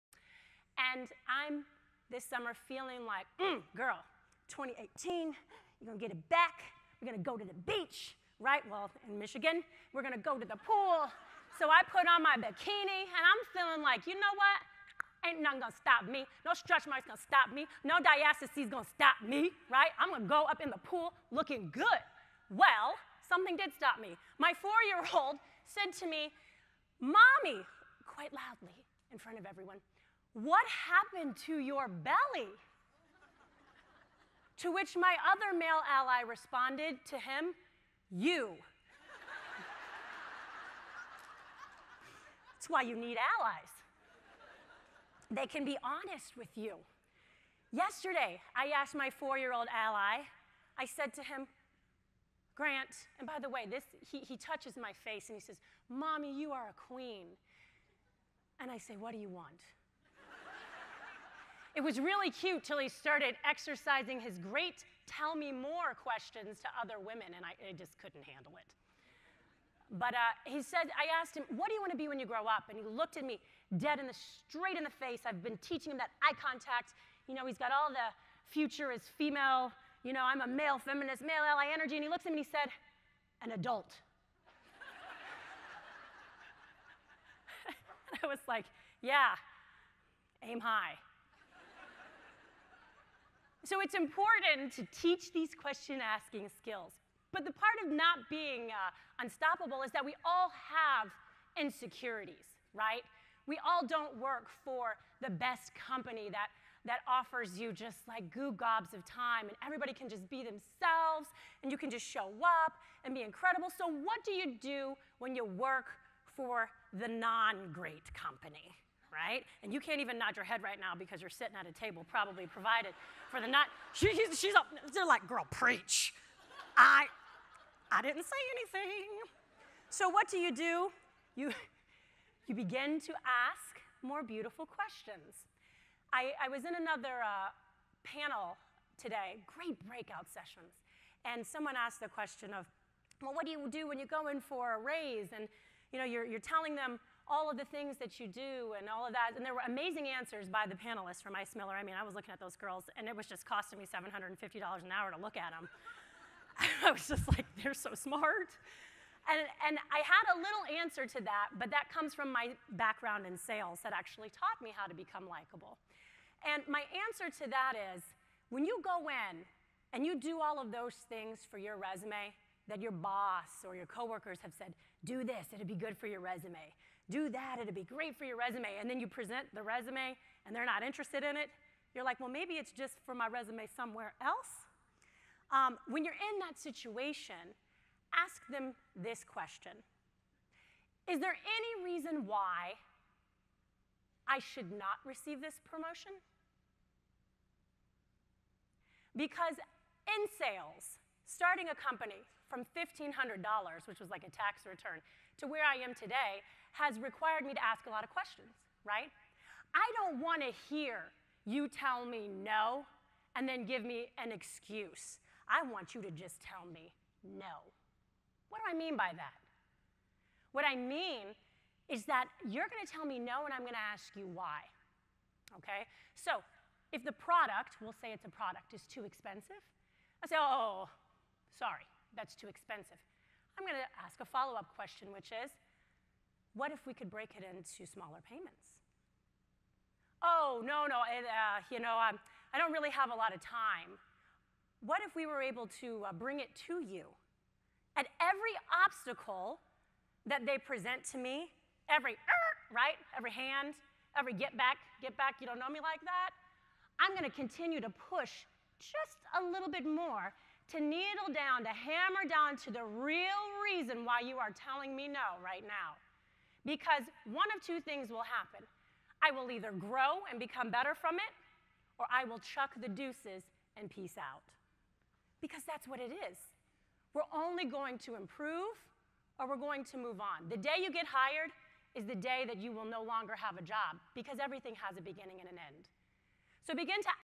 Male Allies And How To Get A Raise | Jennifer Magley
Have you ever felt you should have received a promotion at work?

As E.E. Cummings once said, "A beautiful answer who asks a more beautiful question." Questions are the key to getting exactly what you want. Remember to ask:

"Is there any reason why I should not receive x?" in order to overcome the obstacle that is standing in your way. The obstacle is often "the way" to the next step.

Closing Keynote footage courtesy of the Integrating Women Leaders Conference in Indianapolis, IN.

to continue the conversation.